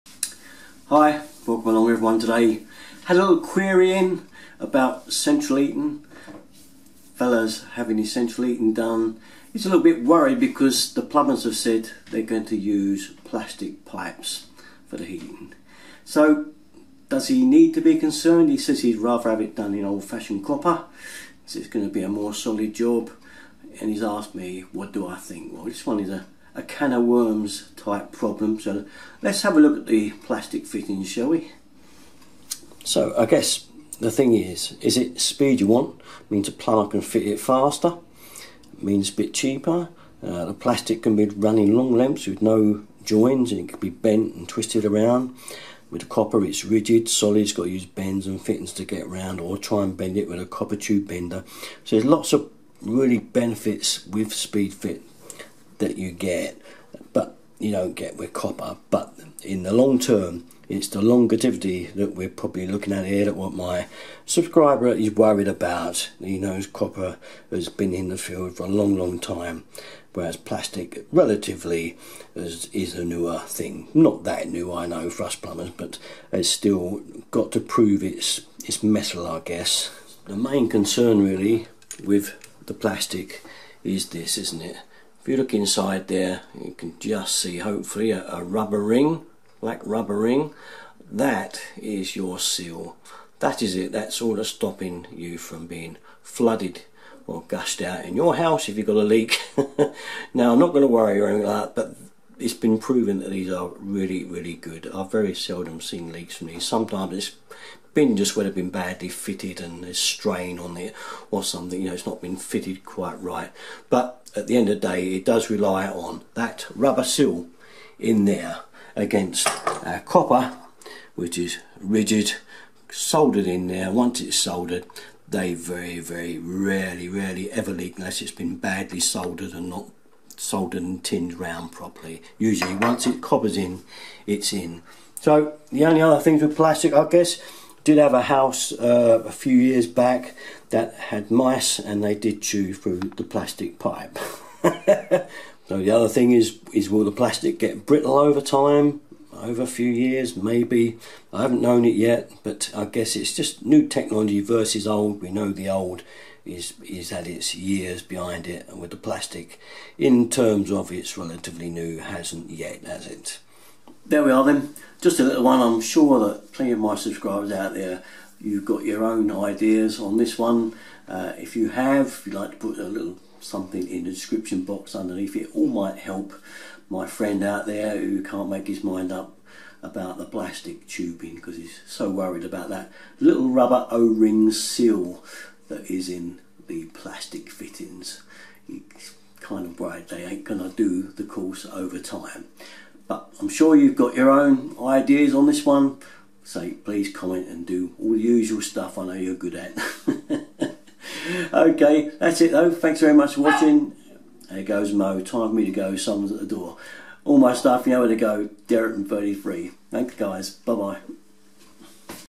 Is it better to use plastic pipes instead of copper ones ?
He is worried about his central heating being run in plastic instead of copper pipe.
Many thanks Al and Jan.